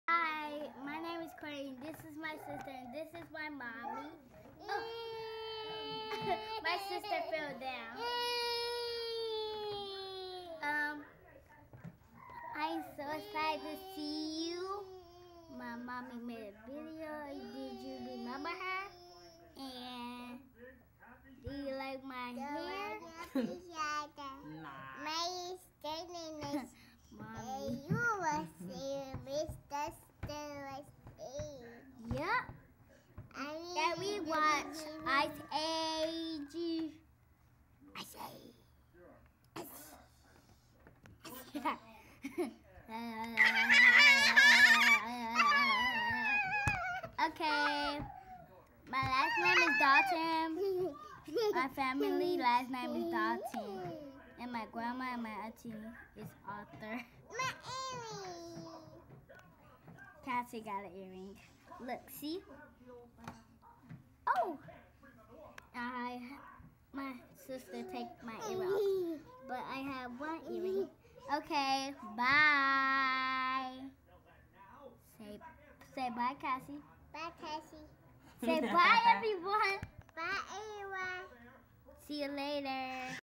MY CHILDREN: KOURTNEY AND KASSIDY
THEY WANTED TO SAY HELLO TO YOU ALL!